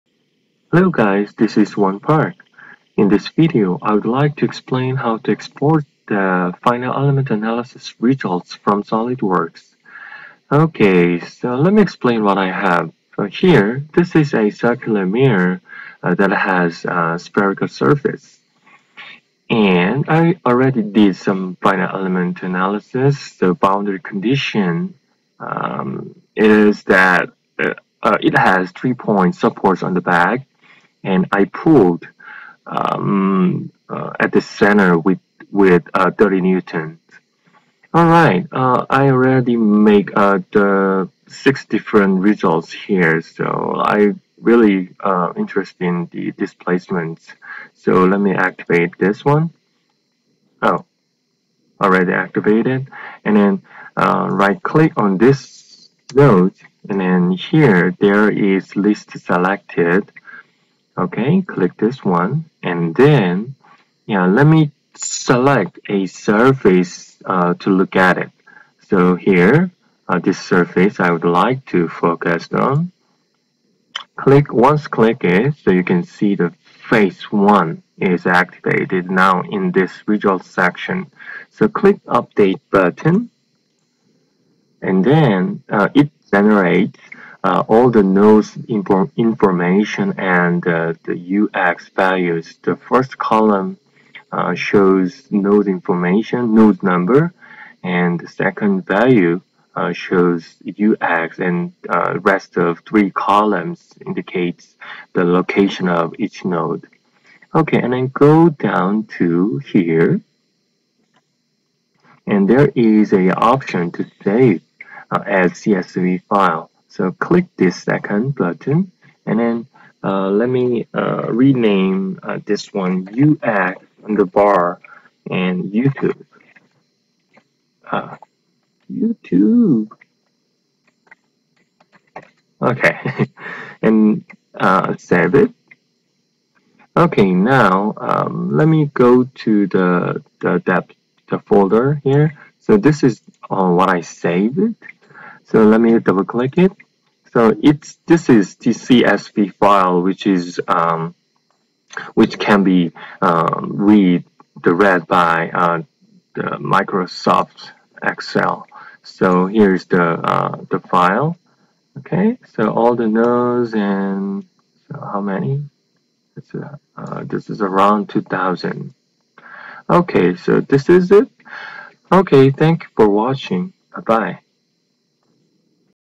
Solidworks Simulation: How to export FEA results
how to export FEA results from solidworks and save it as CSV file format.